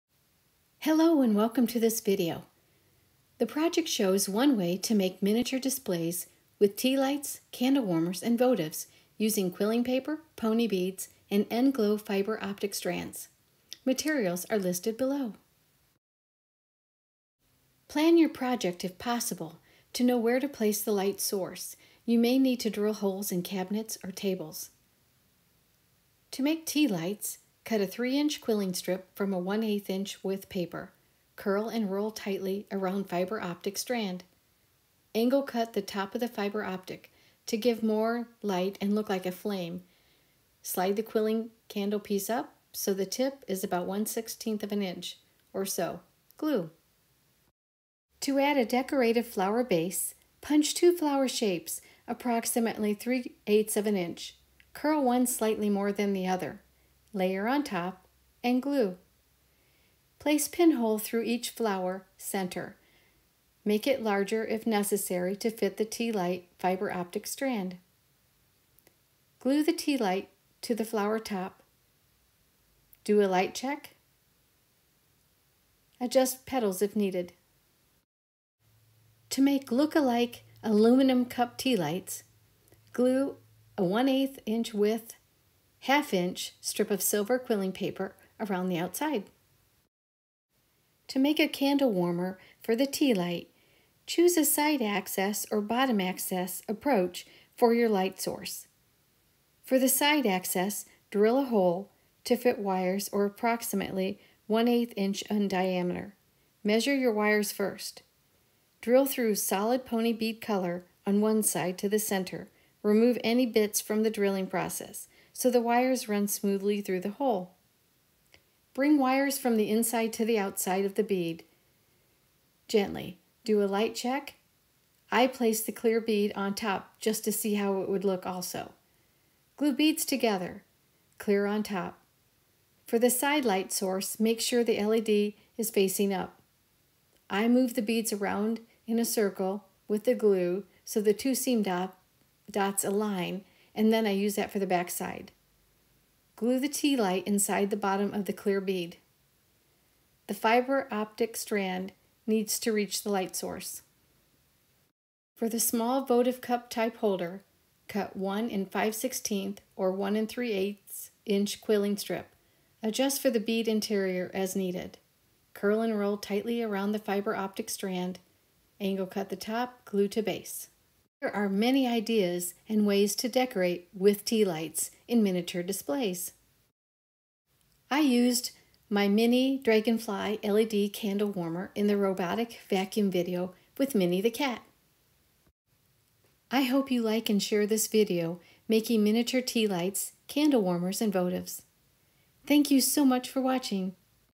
1:12 scale DIY, Easy, dollhouse miniature project making tea lights & candle warmers, LED ready
This is a 1:12 scale DIY, Easy, dollhouse project making tea lights, candle warmers & votives LED ready using quilling paper, pony beads & fiber optic strands.

The video shows one way to make tea lights, candle warmers & votives for dollhouse miniature.   It is helpful to consider locations for display so you can plan on the light source access & set-up.

Materials List:

1. quilling paper - white, cream, silver or your choice in 1/8 inch strips, (3 inch piece for each tea light & 1 3/8 inch piece for small votive tea light) I used 1/4 silver strips for aluminum cup wraps.

2. tea lights: cut ten 3 inch white pieces, votives: cut two 1 3/8 inch white pieces (adjust for bead interior)

3. tea light exterior: two 1/2 inch silver pieces to look like aluminum cups, 1/4 quilling strip width

4. fiber optic strands - thin about diameter size of quilter or sewing pin, about four inches, end flow filament, 0.25 to 0.5 mm from evandesigns.com or your choice. The strands are strong but can bend or snap, use gently.

5. pony beads - two 6 x 9 mm with 4mm hole for each candle warmer: one clear & other a solid color; or one clear  4 x 7 mm with 2.5mm hole for votive; (need two clear 6 x 9 & two solid and two clear 4 x 7)

6. light power source - lighting kit with warm white chip or nano, compact switch & coin cell holder from evandesigns.com or your choice. Note:  The tea light, candle warmers or votives display area will need an access point below base to light through fiber optic strand. If on a countertop, table, shelf or window sill, drill a hole to place candle over & place light power source under in a drawer or cabinet or the candle warmer back side drilled hole access point to insert. The number of kits depends on your display. The company website has useful videos.

7. optional base ideas - card stock, silver quilling paper strip, pony bead, other beads, wood   The bottom will need to be open so fiber optic strand can receive light.

8. glue - PVA clear drying, E6000 Bead & Jewelry glue

9. toothpicks - for gluing

10. containers - like plastic lids to hold glue

11. clamps

12. tweezers

13. craft knife & mat & scissors

14. stylus & foam piece

15. drill & bit - small, to go through pony bead or base side

16. ruler & pencil

Optional accessories:

a. copy paper - to punch or cut decorations

b card stock - to punch base decor like flower shape in assorted colors, approximately 2 x 3 inch piece for two flower piece color

c. acrylic paints - to color decorations or candles

d. paper punches - shapes for decorations or candle base, small flower approximately 3/8 inch diameter, two pieces each, see video

e. stickers - ideas for decorations: dragonflies, butterflies, flowers, leaves, suns, stars, hearts & many others, may need to apply with super glue or bead glue

f. permanent markers - to color decorations or candles, or color flame end of fiber optic (I did not color ends)

g. jewelry findings & bits - jump rings that can be removable decor

h. artificial snow - decorations, color with paints

i. no hole bits

Notes:
*supplies & materials available online or at your local arts & crafts stores, large department stores or miniature shows
*sizes are close approximations
*the tea light, candle warmer or votive display area will need an access point below pillar to light through fiber optic strand. If on a countertop, table, shelf or window sill, drill a hole to place candle over & place light power source under in a drawer or cabinet.

Uses ideas:
*1:12 miniature diorama displays, stores, shops, kitchens, room boxes, living & family rooms, bedrooms, bathrooms, offices & gifts for special events
*club projects or activities - non commercial purposes

…Look for my upcoming 1:12 scale projects…including more from the wildflower series and spice holders with containers…

Cathy
Miniaturist